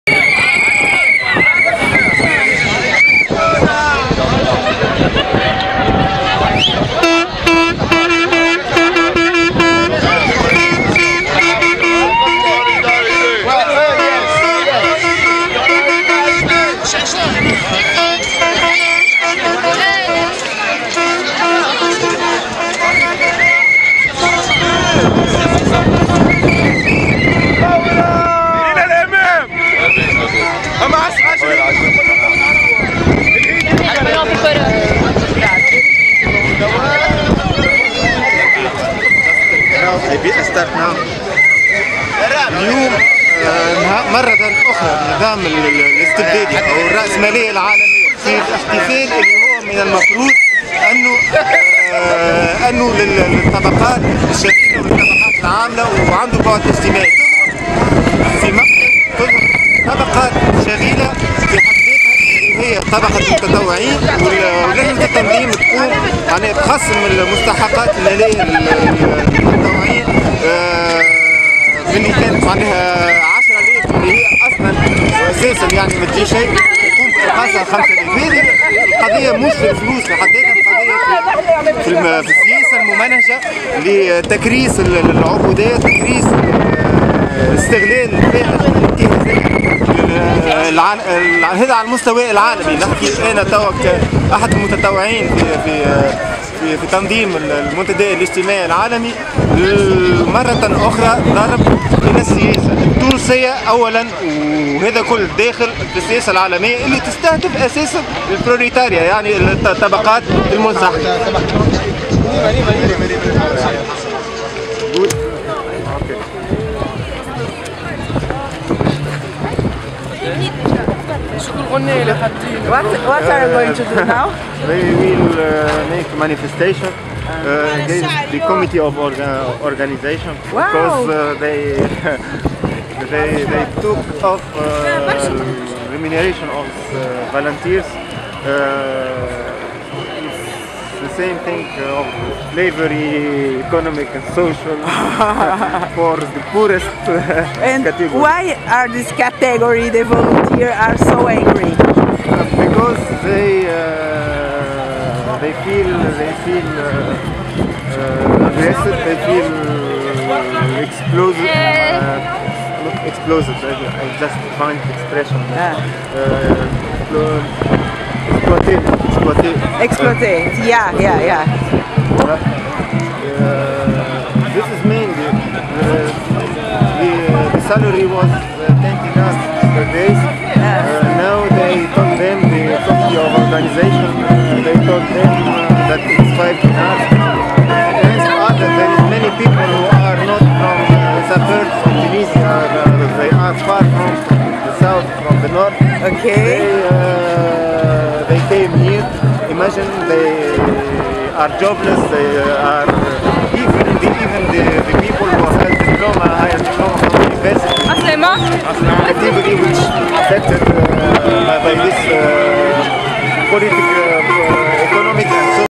Voluntários do Fórum Social Mundial entram em greve
Assembleia realizada nesta sexta-feira (27) decretou a greve dos voluntários do Fórum Social Mundial em Túnis por falta de pagamento e más condições de trabalho